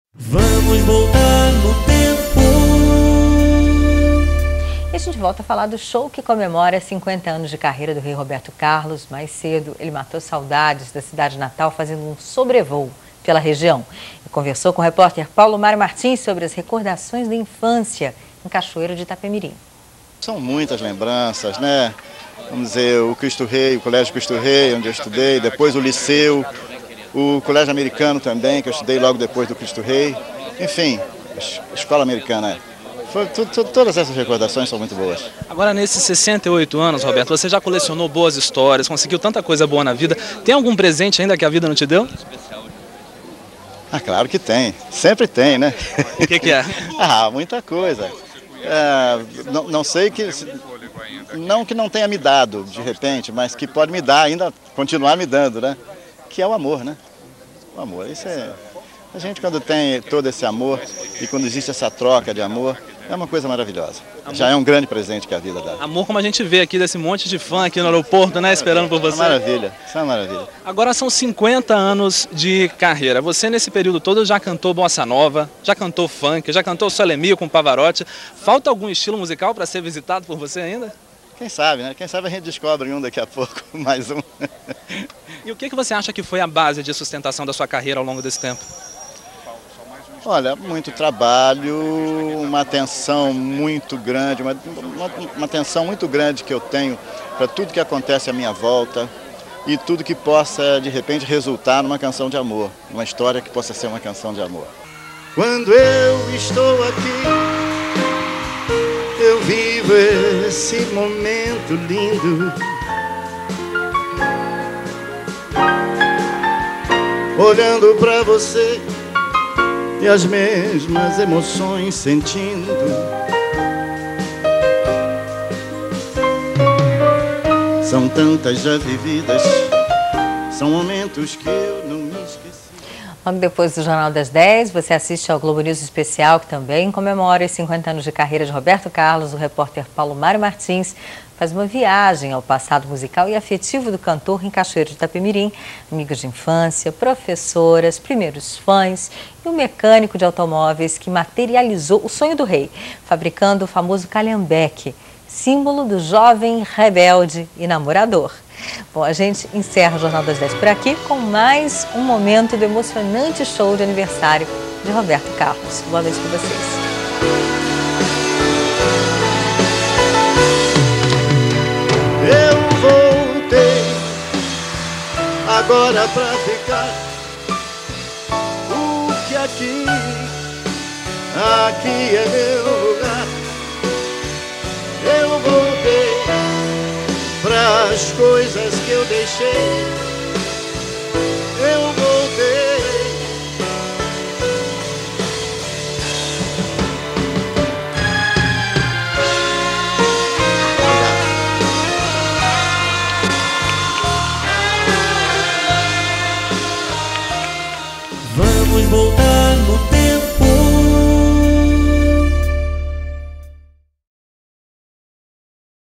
Roberto Carlos - 50 Anos de Carreira
Roberto Carlos comemora os seus 68 anos de idade e 50 anos de carreira em sua cidade natal. Cachoeiro de Itapemirim.